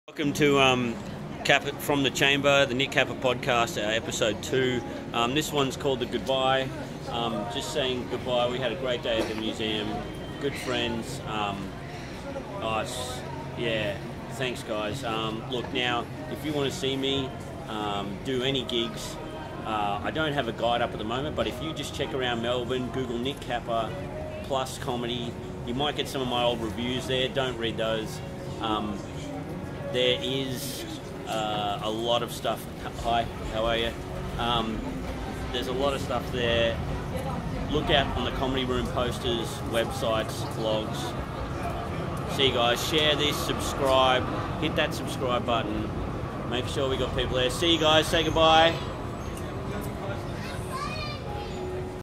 VLOGAPPER (Nick Capper's video blog(a vlog) Episode 2
Nick wraps up his day with his mates